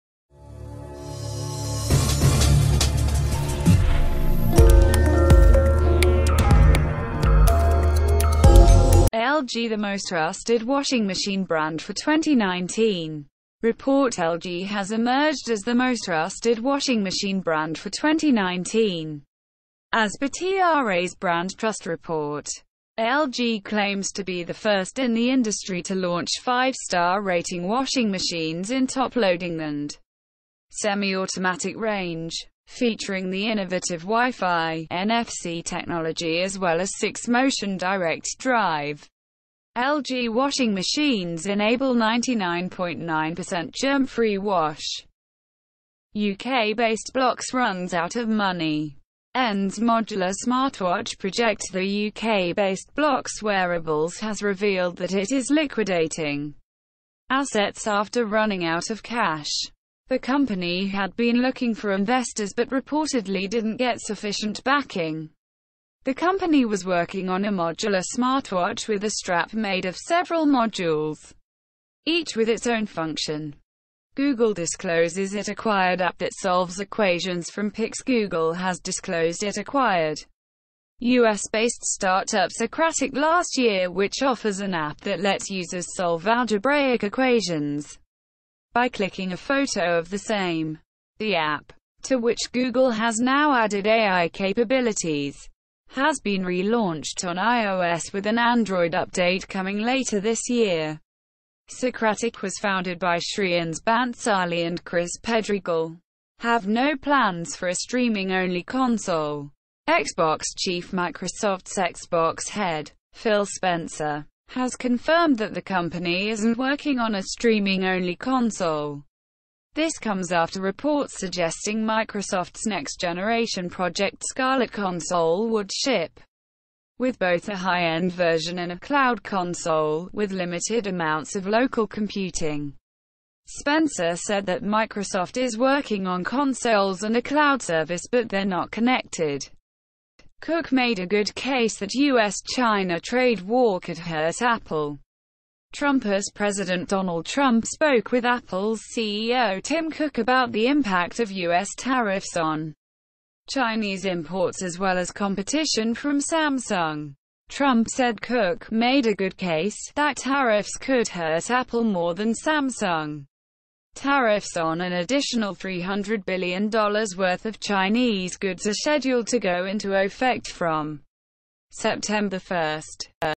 Latest Technology News - LG the most trusted washing machine brand for 2019: Report
--- LG the most trusted washing machine brand for 2019: Report ---
  LG has emerged as the most trusted washing machine brand for 2019, as per TRA's Brand Trust Report. LG claims to be the first in the industry to launch 5-star rating Washing Machines in Top Loading & Semi-Automatic range. Featuring the innovative Wi-Fi/NFC technology as well as 6-Motion Direct Drive, LG washing machines enable 99.9% germ-free wash.
 --- UK-based Blocks runs out of money, ends modular smartwatch project ---
  The UK-based Blocks Wearables has revealed that it is liquidating assets after running out of cash. The company had been looking for investors but reportedly didn't get sufficient backing. The company was working on a modular smartwatch with a strap made of several modules, each with its own function.
 --- Google discloses it acquired app that solves equations from pics ---
  Google has disclosed it acquired US-based startup Socratic last year which offers an app that lets users solve algebraic equations by clicking a photo of the same. The app, to which Google has now added AI capabilities, has been relaunched on iOS with an Android update coming later this year. Socratic was founded by Shreyans Bhansali and Chris Pedregal.
 --- Have no plans for a streaming-only console: Xbox Chief ---
  Microsoft's Xbox head, Phil Spencer, has confirmed that the company isn't working on a streaming-only console. This comes after reports suggesting Microsoft's next-generation Project Scarlett console would ship with both a high-end version and a "cloud console" with limited amounts of local computing. Spencer said that Microsoft is working on consoles and a cloud service but they're not connected.
 --- Cook made a good case that US-China trade war could hurt Apple: Trump ---
  US President Donald Trump spoke with Apple's CEO Tim Cook about the impact of US tariffs on Chinese imports as well as competition from Samsung. Trump said Cook "made a good case" that tariffs could hurt Apple more than Samsung. Tariffs on an additional $300 billion worth of Chinese goods are scheduled to go into effect from September 1.